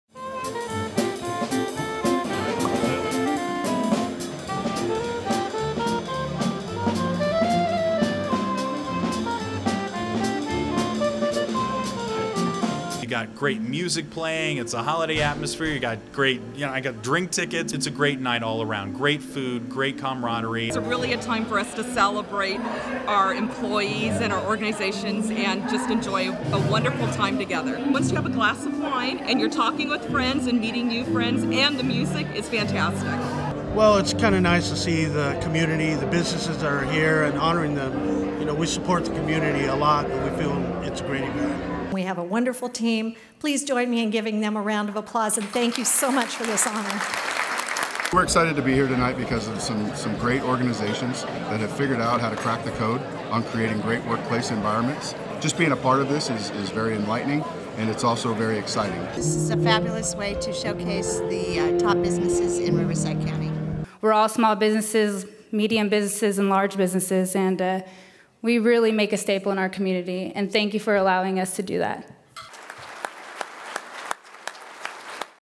The Riverside Press Enterprise's Top Workplaces for 2015
Nearly 300 local executives and business leaders joined Monster.com, Workplace Dynamics and The Press-Enterprise in celebrating the Inland Empire’s best places to work at the Riverside Convention Center.  Special thanks to all the 2015 Top Workplaces sponsors and honored companies.